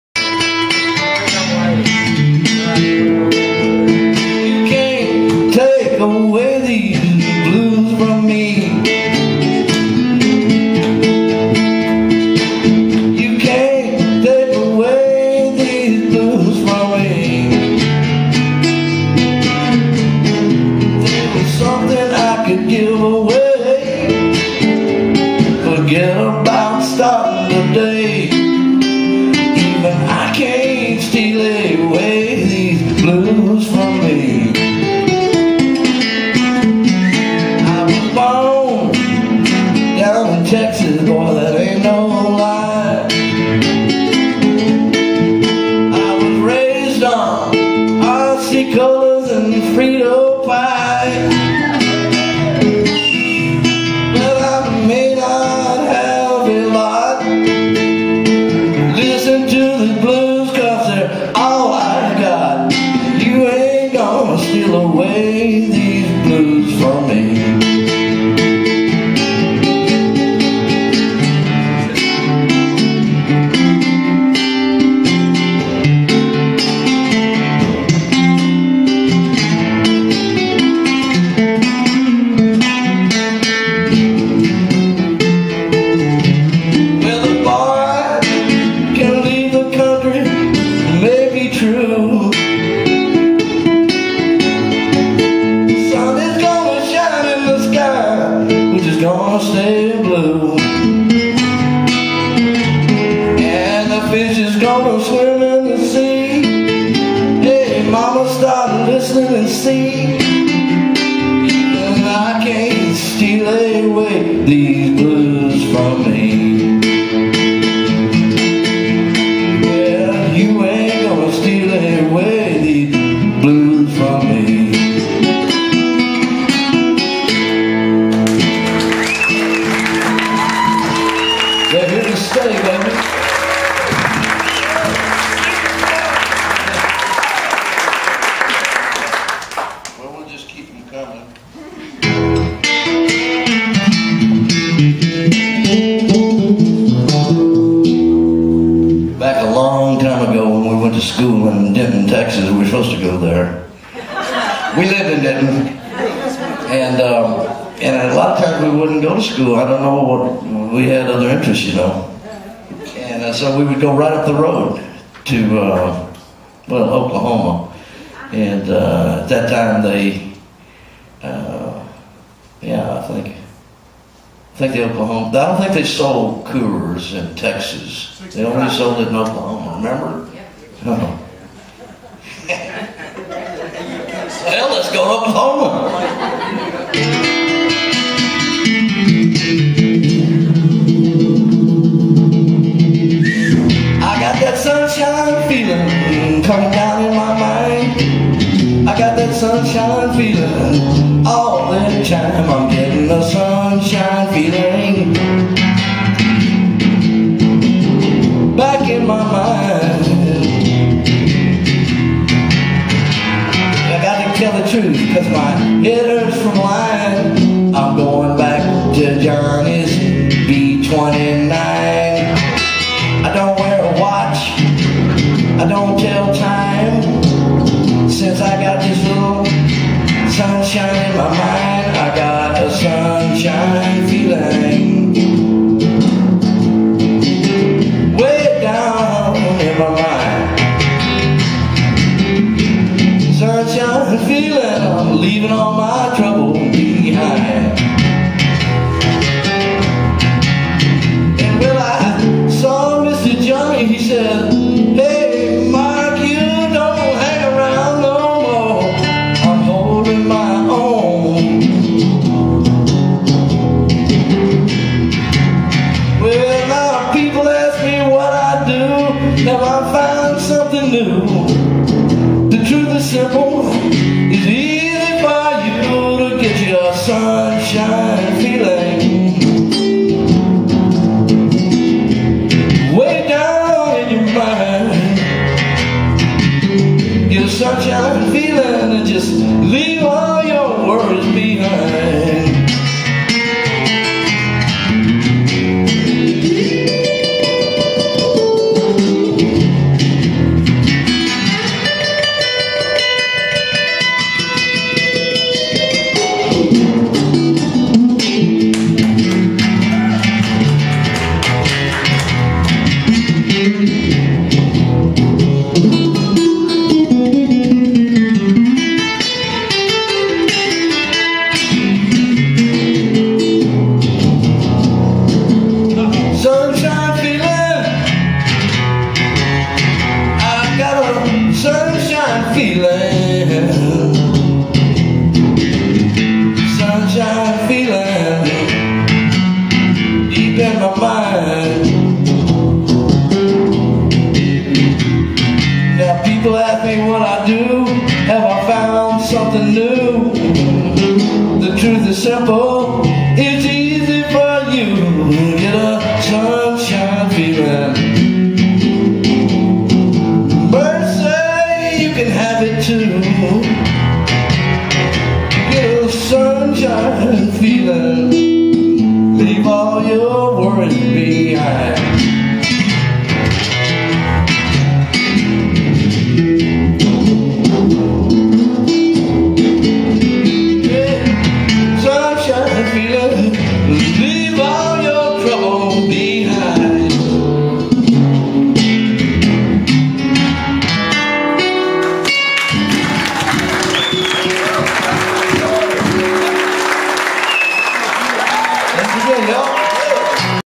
Marc Benno in Dallas, Texas
Recorded live at the historic Kessler Theater in X+ North Oak Cliff. (June 4, 2011)